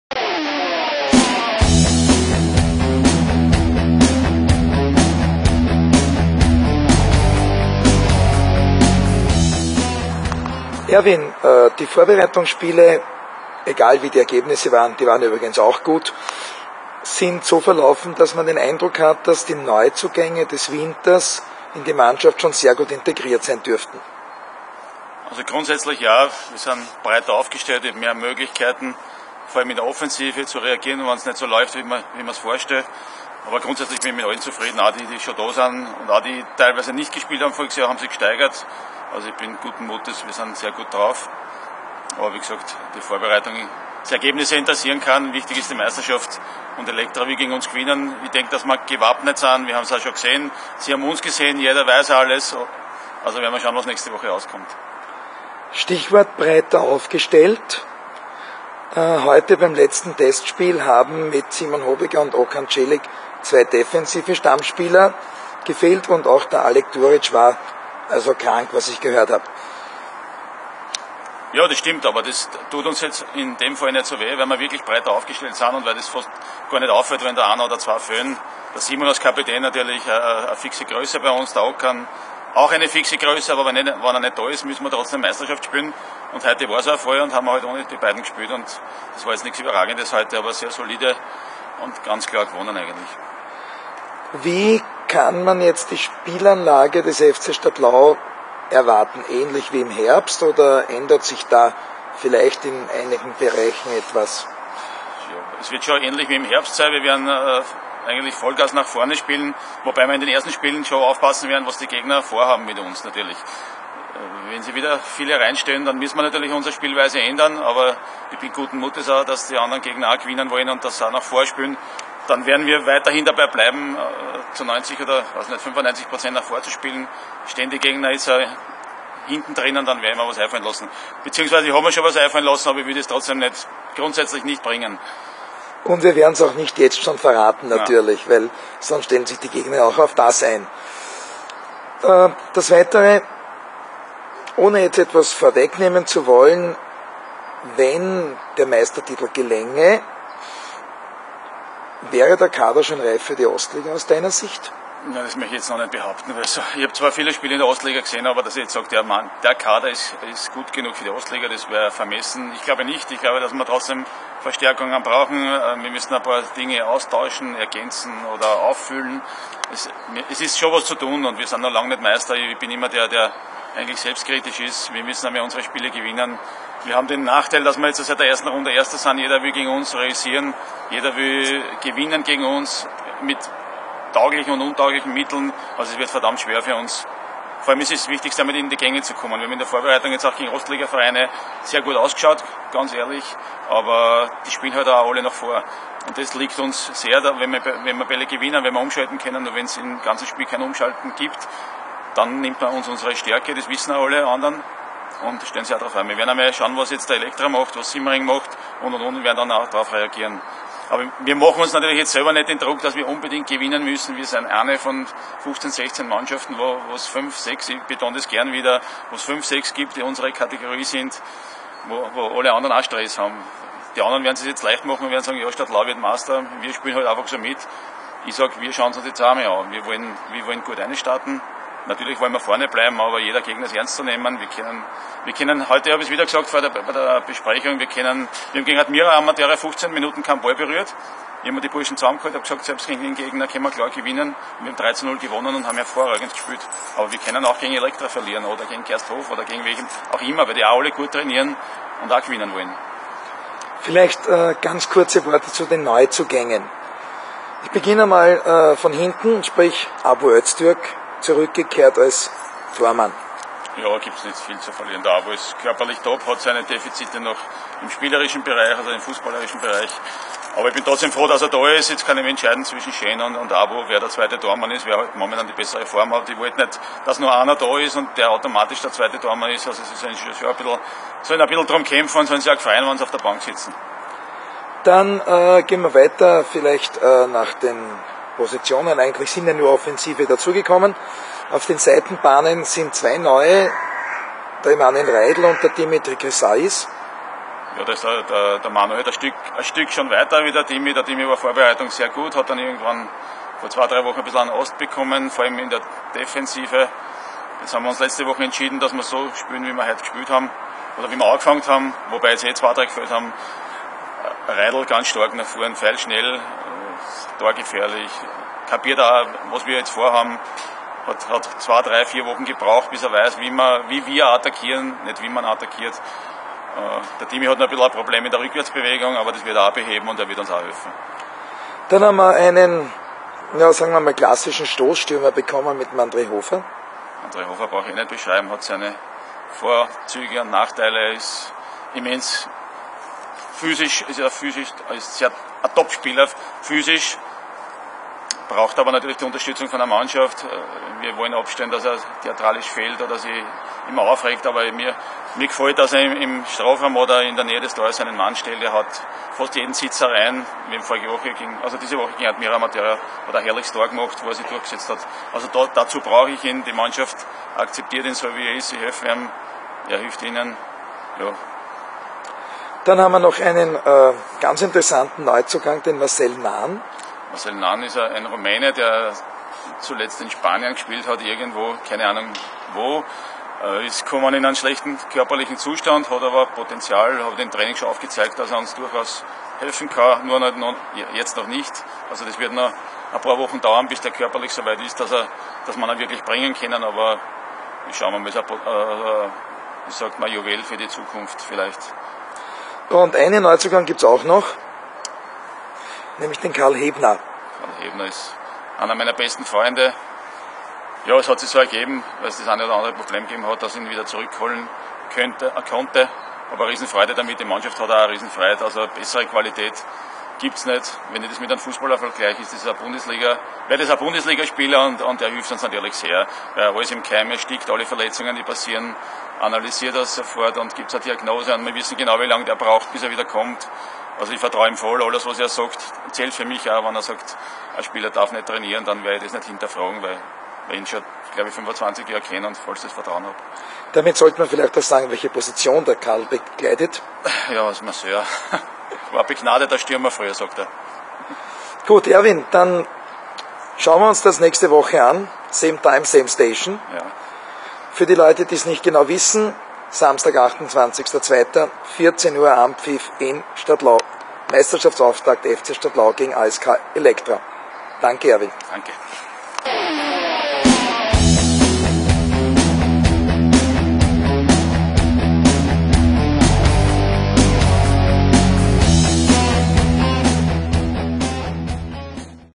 Stadlau-Web-TV vom 21.2.2015: Erwin Cseh
... im Interview zum Rückrundenstart der Wiener Stadtliga im Frühjahr 2015